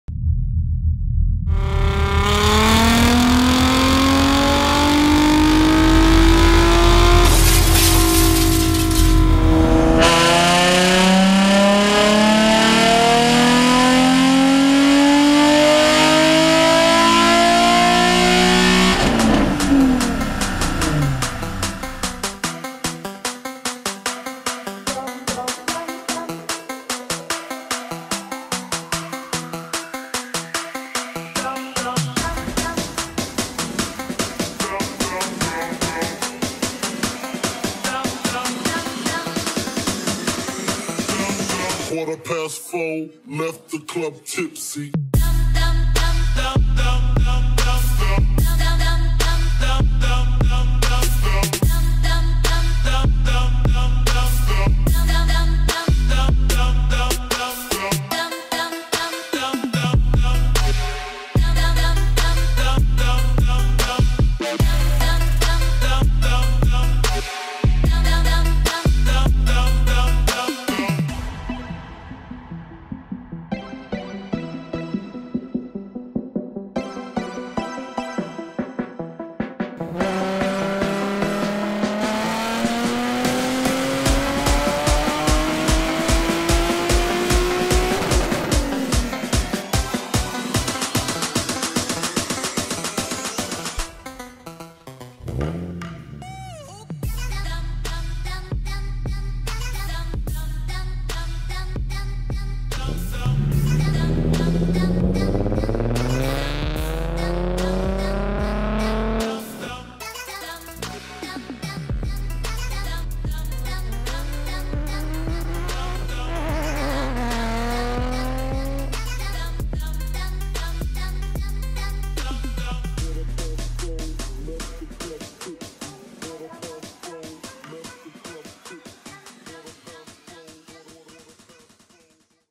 Tomei Expreme Titanium Exhaust 350z Install - Z1 Motorsports
350Z Tomei Expreme Titanium Exhaust Testing at Z1 Motorsports.  Available for 350Z and G35 Coupe Models.